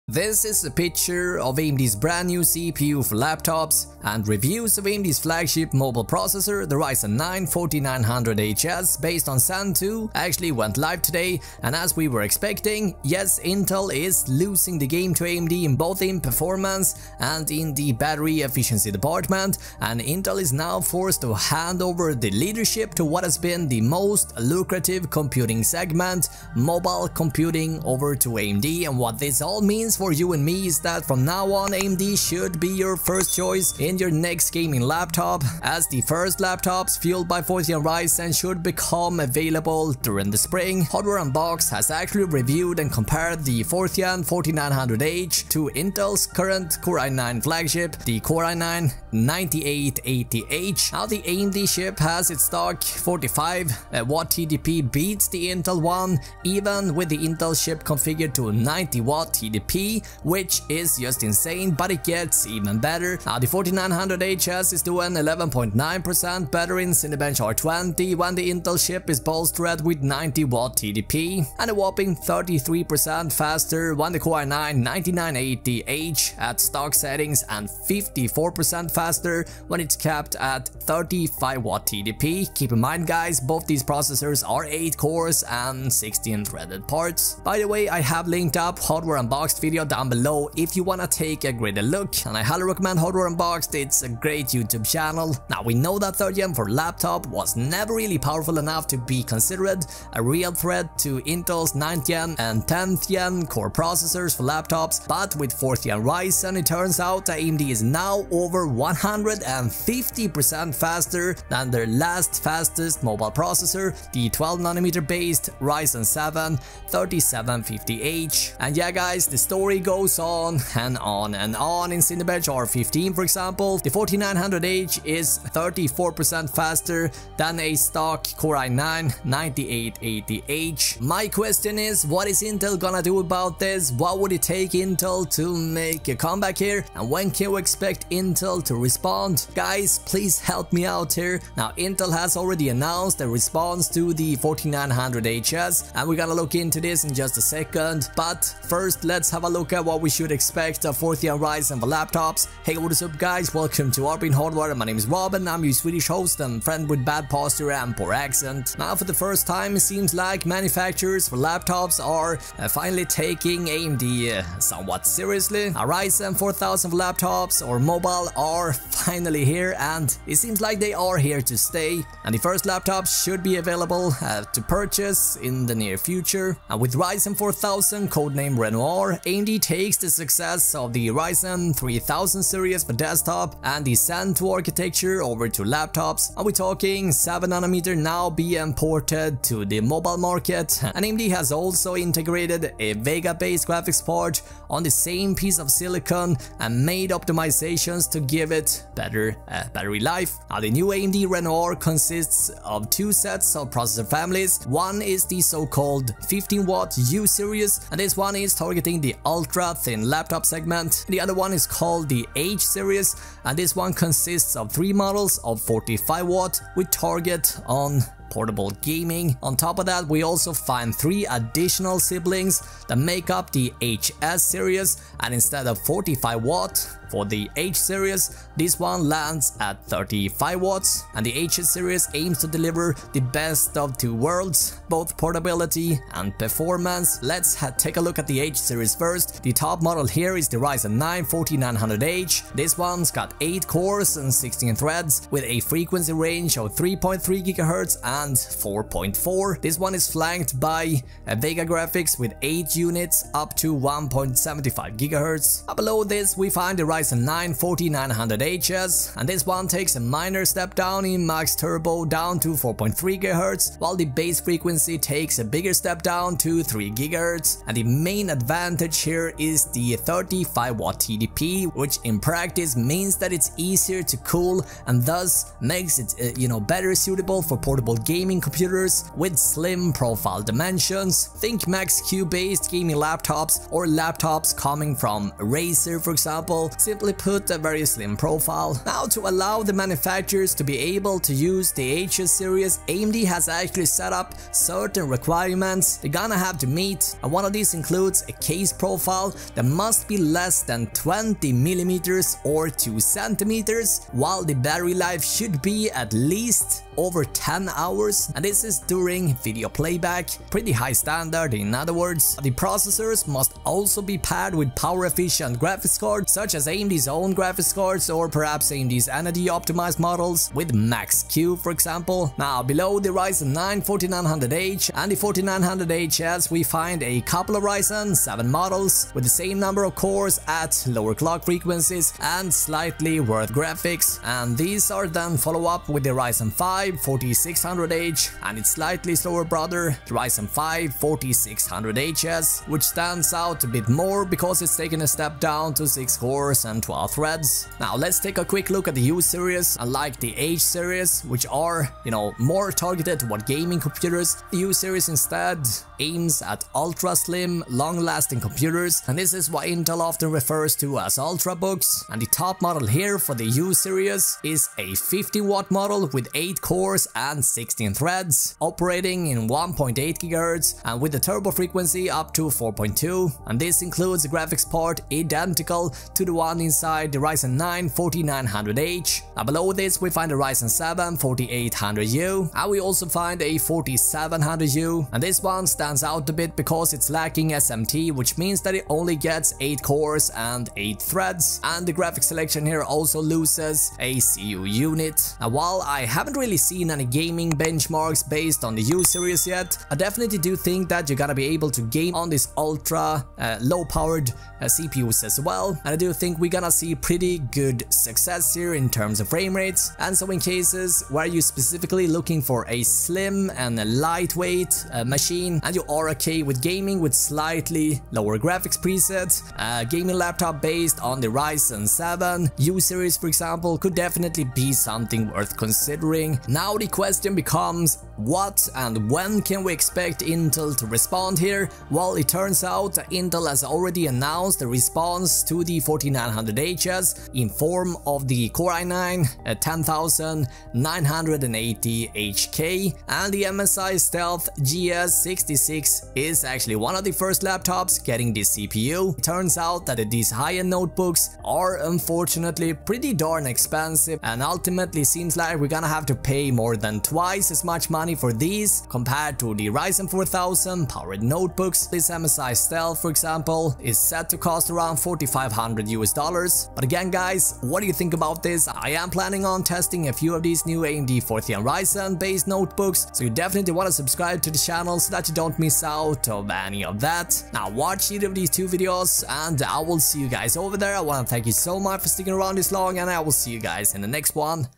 AMD Ryzen 4000 Has Finally Landed & It’s UNSTOPPABLE! INSANE Specs & Performance!
AMD's new Ryzen 4000 APUs are amazing, with the Ryzen 9 4900HS beating all intel cpu offerings. Today we look at everything you need to know about AMD's new Ryzen 4000 Renoir.